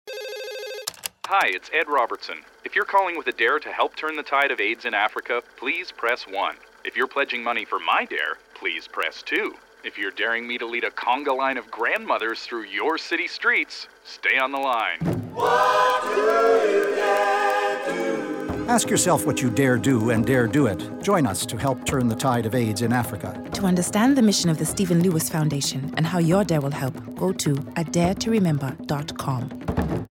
Dare radio spot: Ed Robertson of Barenaked Ladies - Ed's Answering Machine
A DARE TO REMEMBER is a campaign of the Stephen Lewis Foundation -- engaging Canadians to raise funds for AIDS in Africa. In powerful acts of solidarity, Canadians across the country are facing their fears, letting their creative juices flow, testing their endurance and mobilizing their communities.

What's your Dare for AIDS in Africa?